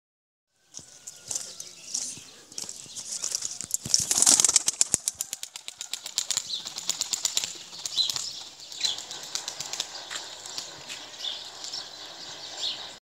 Çakmaklı güvercin şampiyon güvercin
Çakmaklı güvercin şampiyon güvercin deneme ilk kalkış 6 takla taklacı güvercin.